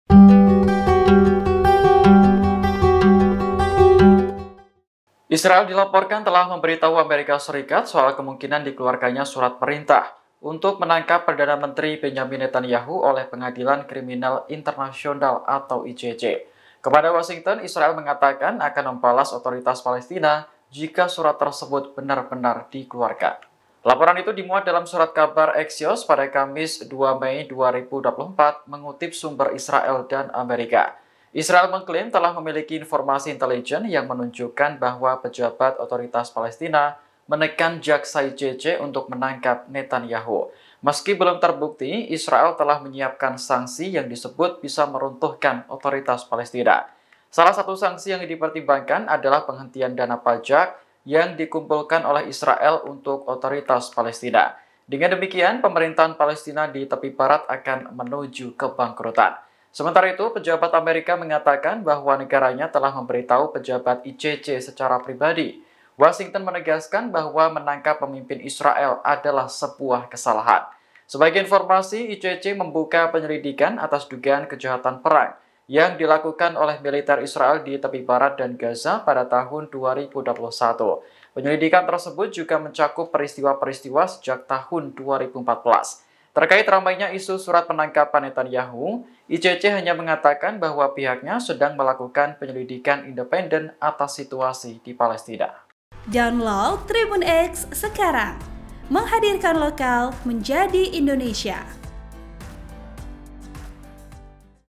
Mengadu ke AS, Israel Ancam akan Buat Palestina Bangkrut Jika ICC Rilis Surat Penangkapan Netanyahu
TRIBUN-VIDEO.COM - Israel dilaporkan telah memberi tahu Amerika Serikat (AS) soal kemungkinan dikeluarkannya surat perintah untuk menangkap Perdana Menteri Benjamin Netantahu oleh Pengadilan Kriminal Internasional (ICC).

Kepada Washington, Israel mengatakan akan membalas Otoritas Palestina (PA) jika surat tersebut benar-benar dikeluarkan.

VP: Dharma